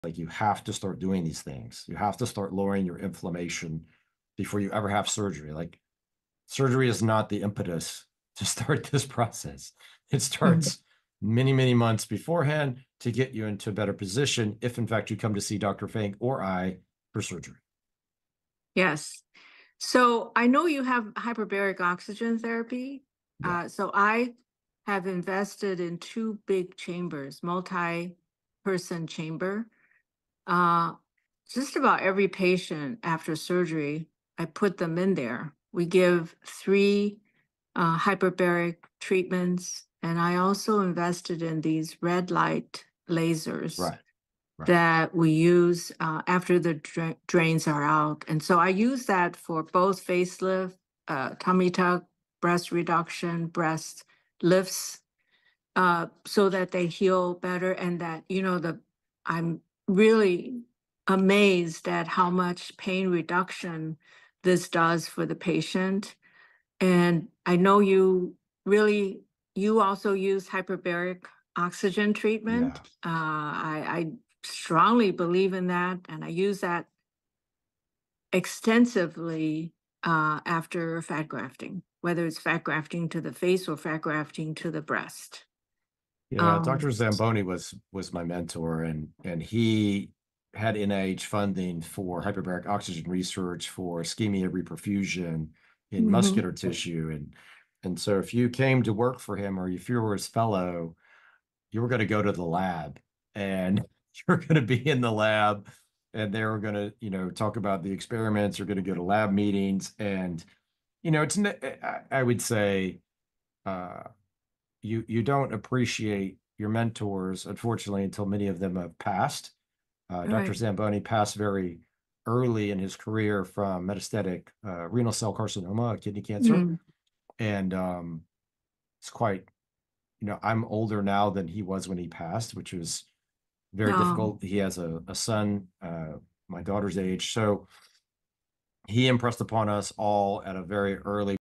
Hyperbaric Oxygen & Surgery: Healing Faster, Less Pain
Discover how hyperbaric oxygen and red light lasers enhance healing and reduce pain after surgery. we discuss pre-surgery inflammation reduction and benefits of hyperbaric treatments.